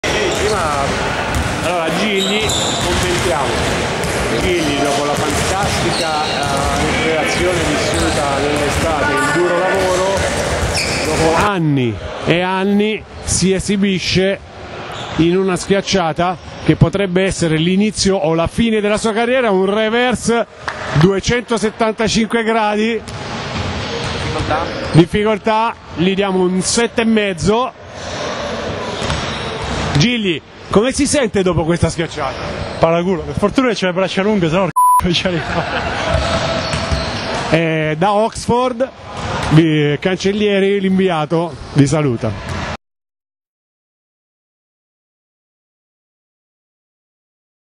La "gara delle schiacciate" di Angelo Gigli
Il centro dell'Olimpia è sulla lunga via del recupero dall'infortunio. Per OlimpiaMilanoTV un "inviato" d'eccezione, coach Cancellieri